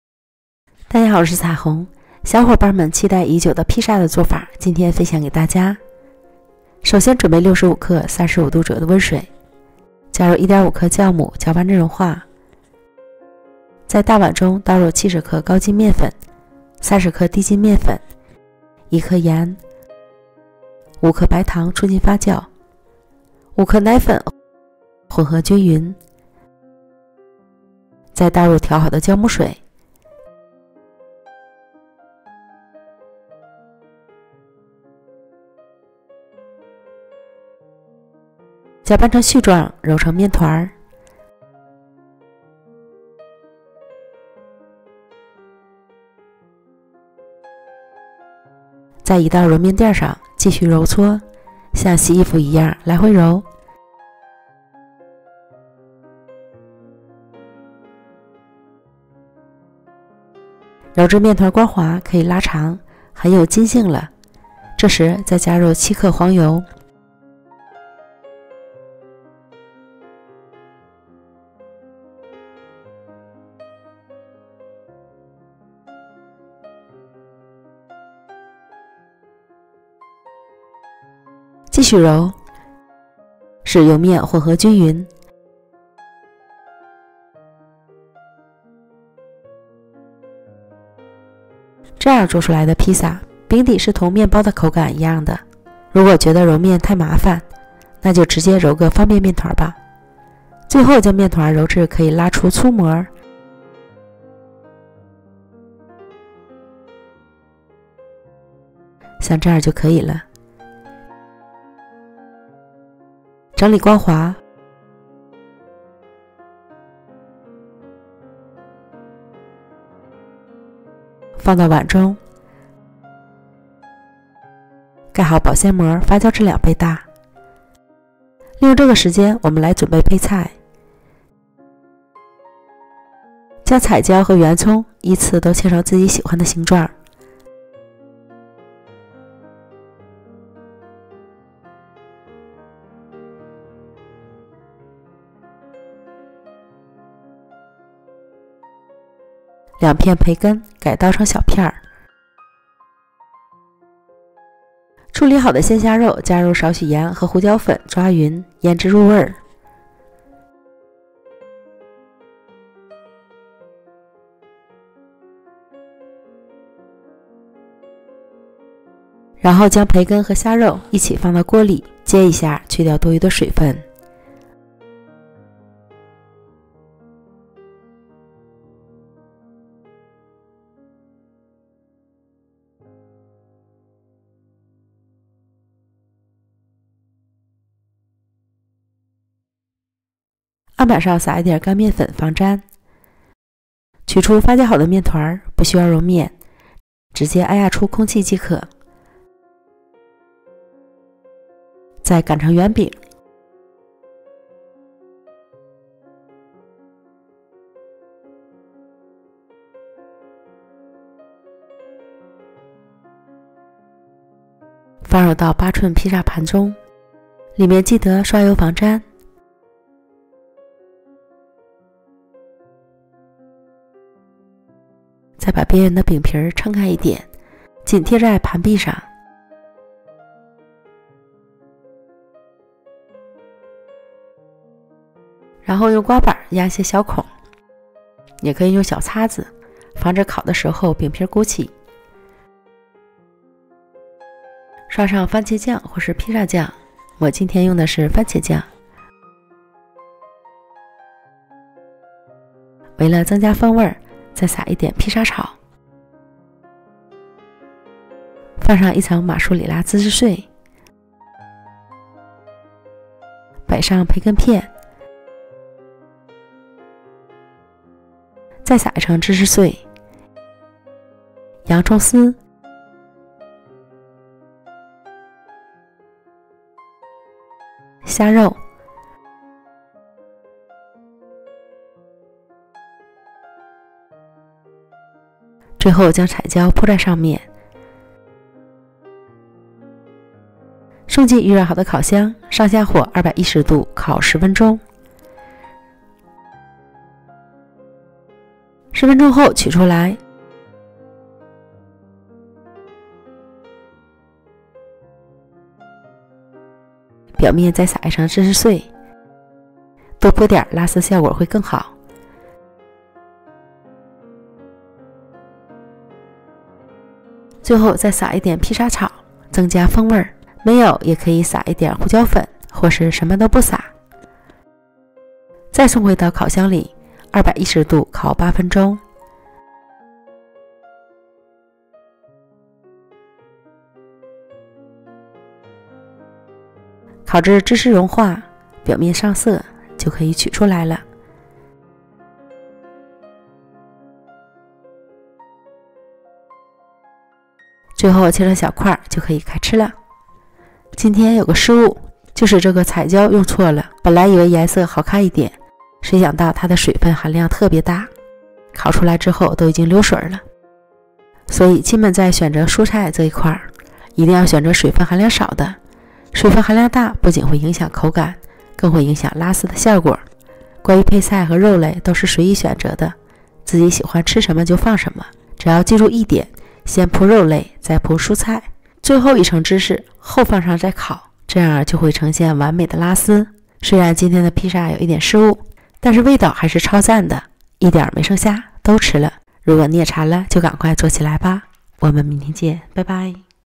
披薩好吃又訣竅，餅底似麵包，拉絲有秘訣，比買的還好吃，真香
饼底部分：
温水65克         盐1克
酵母1.5克        白糖5克
高筋面粉70克     奶粉5克
低筋面粉30克     黄油7克

上层肉菜类按自己喜欢的添加即可

Pizza base parts:
65 grams of salt and warm water 1 g
5 g yeast 1.5 grams of sugar
70 grams of milk powder 5 g
30 g strong flour, butter 7 grams

The upper class meat dishes can be added as they prefer